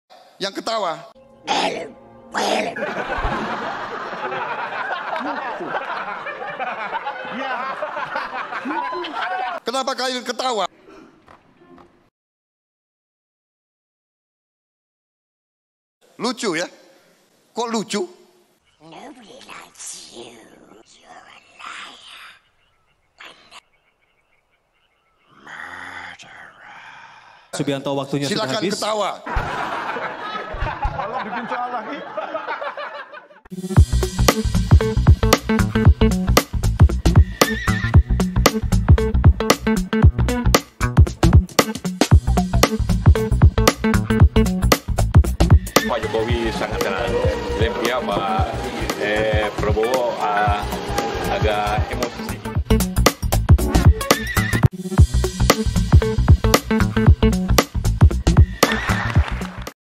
Citra Positif Prabowo Luntur Karena Emosi di Panggung Debat Capres
Analis Politik sekaligus Eksekutif Voxpol Center Research and Consulting, Pangi Syarwi Chaniago menyatakan bahwa dalam debat keempat Pilpres 2019, Prabowo cenderung menampilkan emosi yang tidak terkontrol di sehingga citra positifnya luntur. Prabowo memiliki kontrol emosi yang buruk sehingga sentimen swing voter kembali negatif.

Prabowo arogan, angkuh dan emosional. Indonesia tidak butuh pemimpin emosional karena dampaknya akan bahaya bagi keberlangsungan negara.